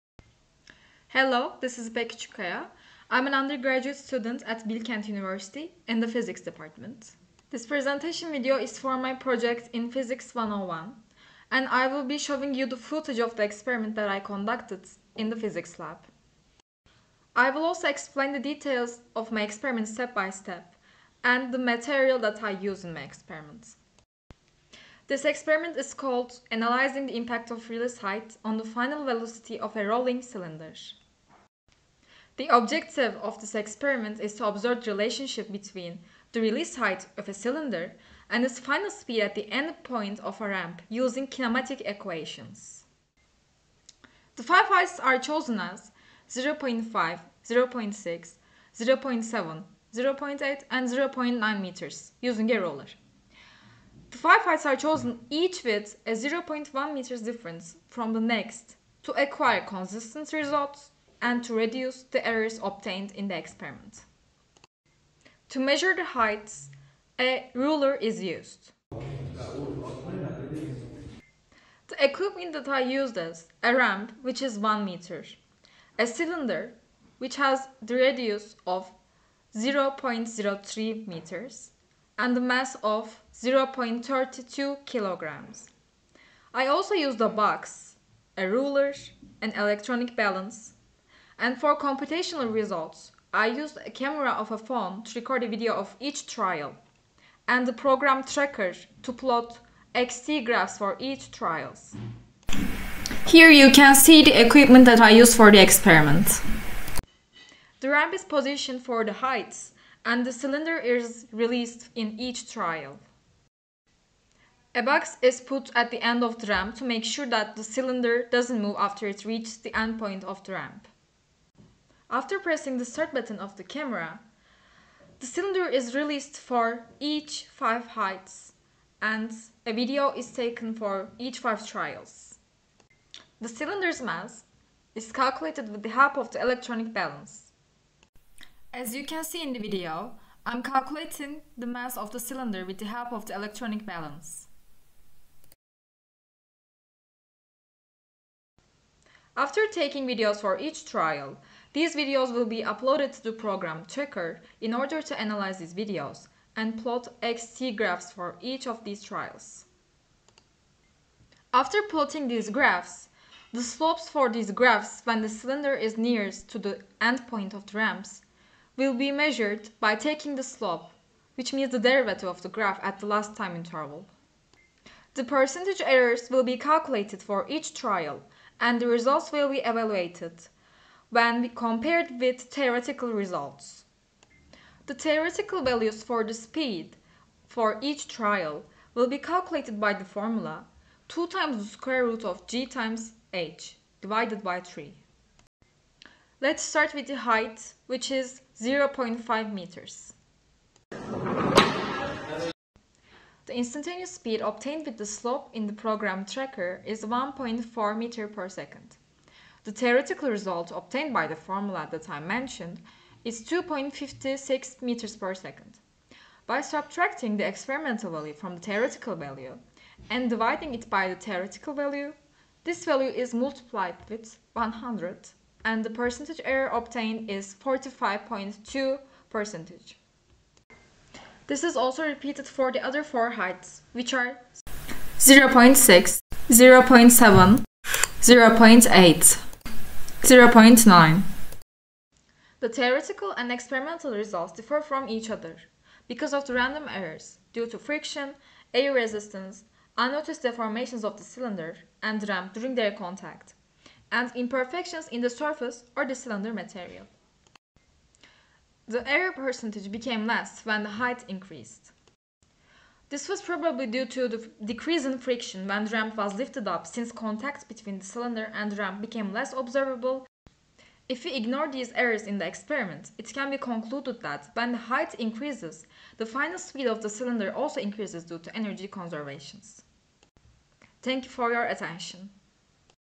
Bilkent Physics 101 Lab Project by İpek Küçükkaya
Hello everyone, in this presentation video, I am describing the experiment that I conducted for Phys101 in Bilkent University.